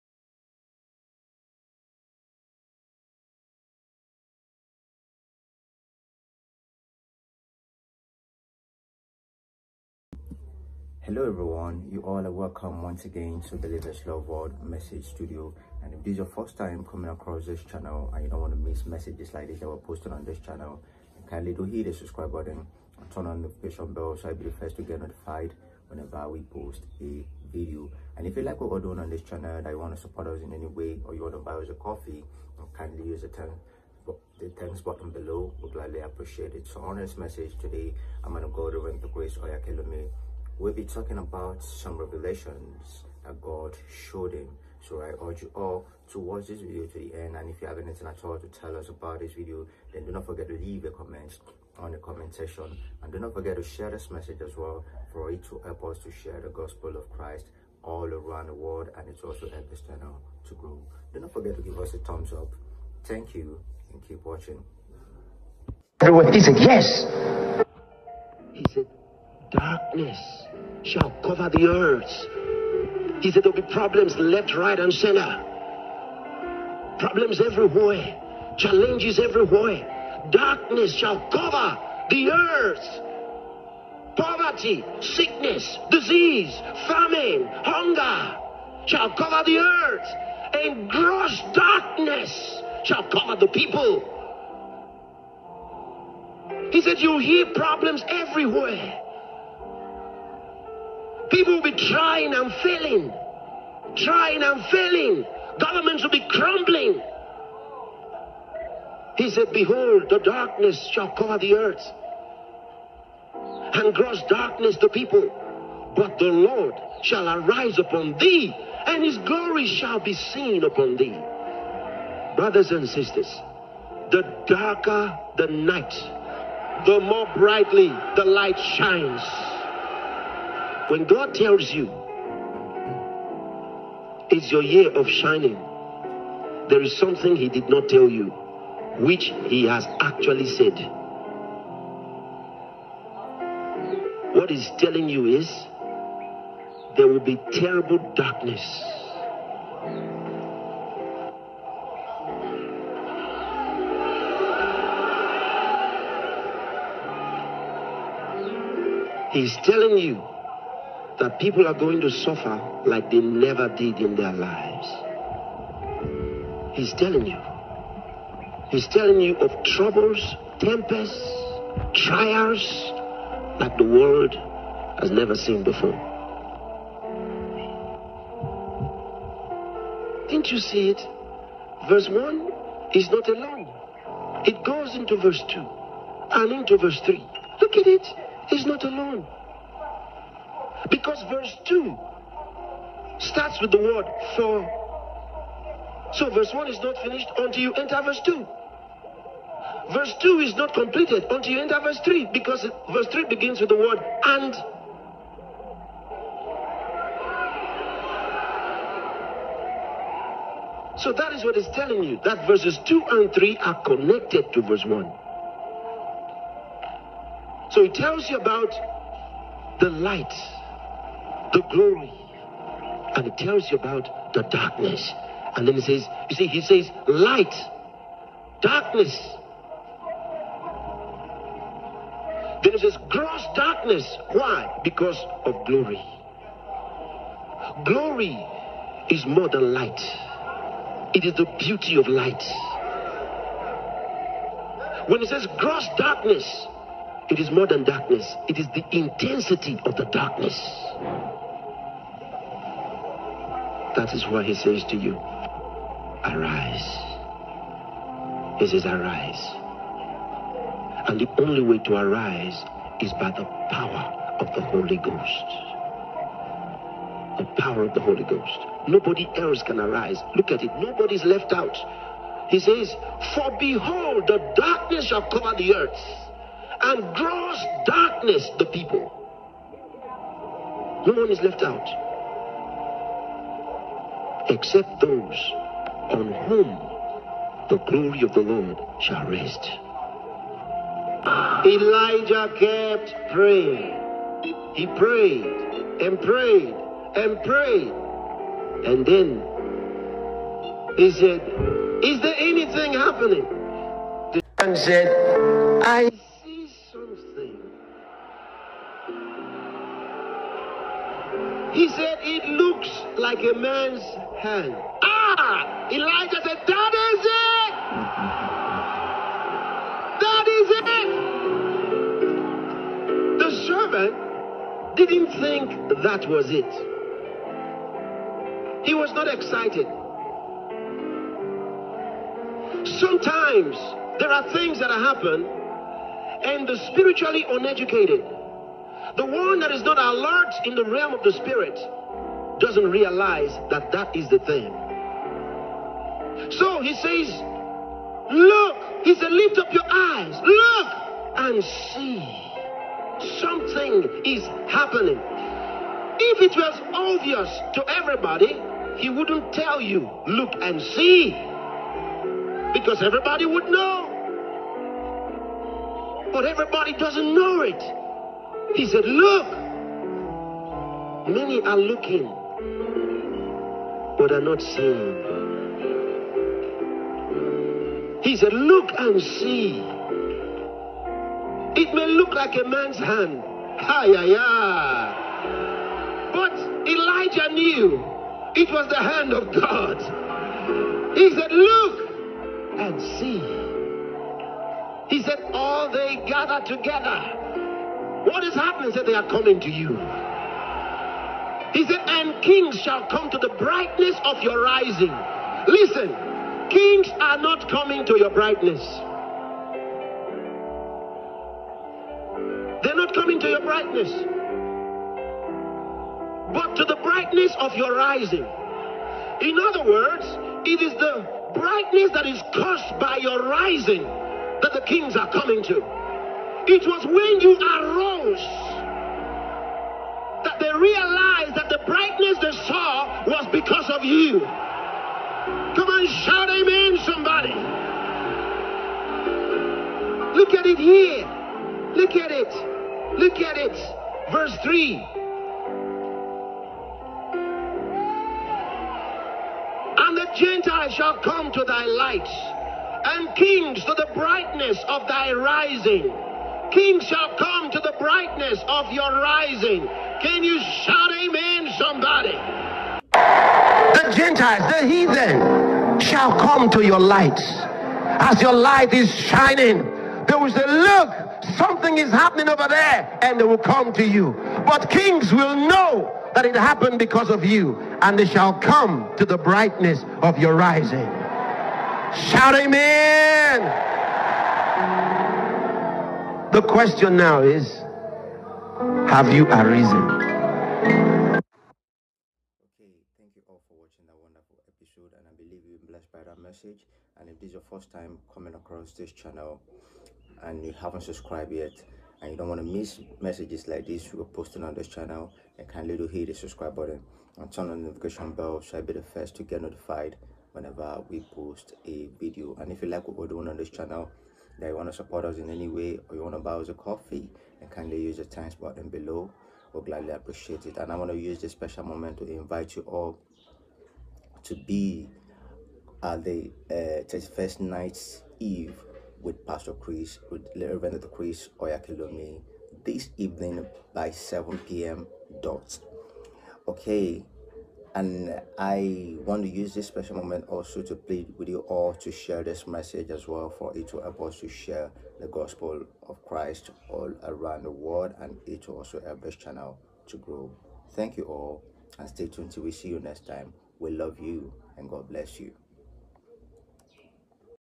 (Reaction) THE REVELATION THE LORD SHOWED ME ABOUT 2024 || PASTOR CHRIS OYAKHILOME DSC, DD
Listen to this insightful teaching by Pastor Chris Oyakhilome pastorchris reaction church god life prayer secret anointing revelation @WatchPastorChris @speakingrhema @WordAlivee